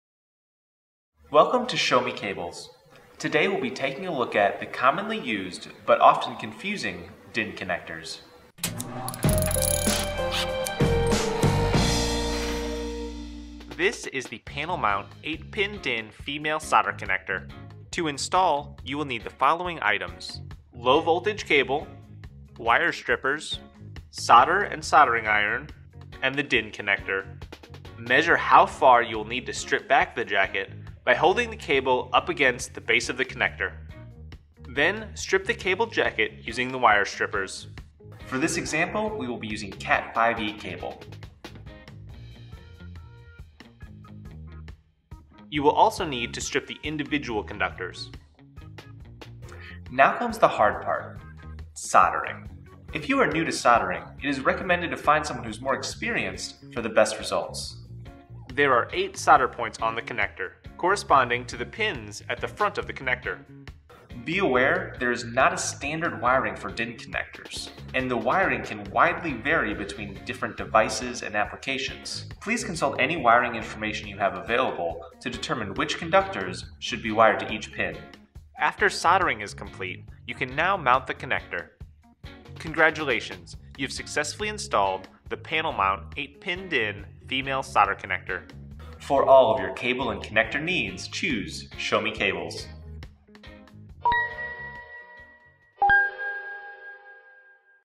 How To Install The 8 Pin DIN Female Panel Mount Connector (270 Degree Style)
Welcome to Show Me Cables.  Today we’ll be taking a look at the commonly used and often confusing DIN connectors.

This is the 8 Pin DIN Female panel mount solder connector.  To install, you will need the following items:  low voltage cable, wire strippers, solder and soldering iron and the DIN connector.

Measure how far you will need to strip back the jacket by holding the cable up against the base of the connector.

Then, strip the cable jacket using the wire strippers.  For this example, we are using Cat5e cable.  You will also need to strip the individual conductors.

Now comes the hard part.  Soldering.  If you are new to soldering, it is recommended to find a someone who is more experienced for best results.

There are 8 solder points on the connector, corresponding to the pins at the front of the connector.  Be aware, there is not a standard wiring for DIN connectors and the wiring can widely vary between devices and applications.  Please consult any wiring information you have available to determine which conductors should be wired to each pin.

After soldering is complete, you can now mount the connector.

Congratulations, you have successfully installed the 8 Pin DIN Female panel mount solder connector.